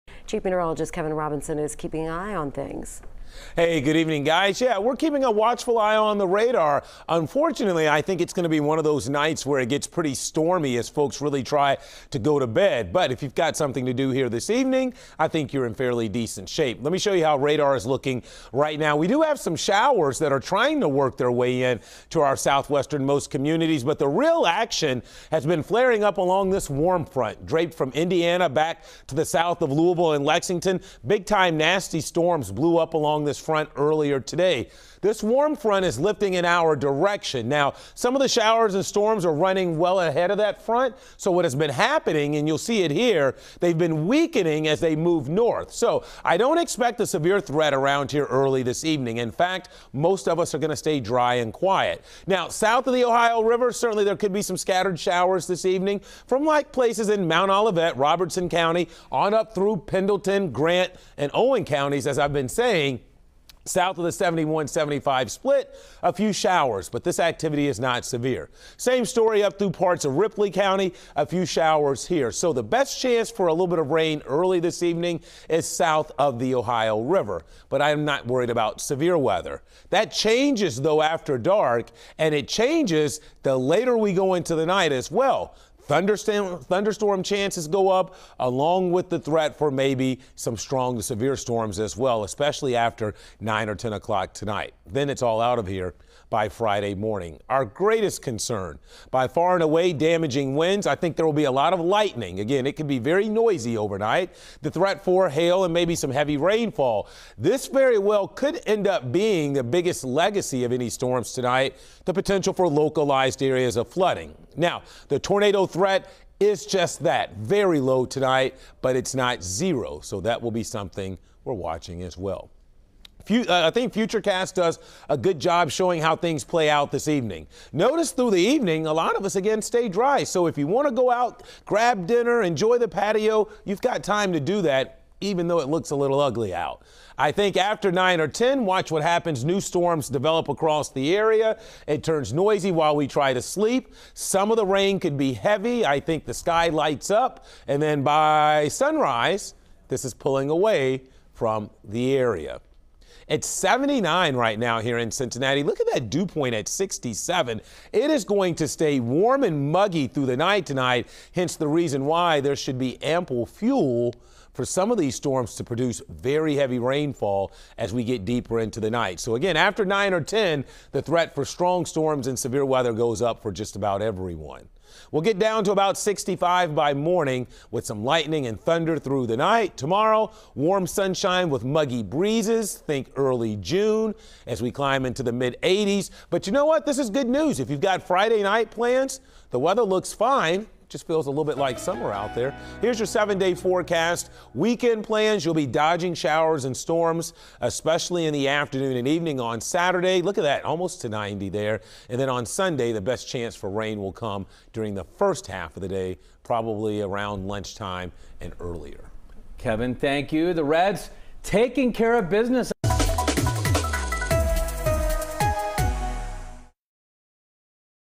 Severe Storms Possible Later Tonight
Scattered strong to severe storms will be possible late evening into the overnight.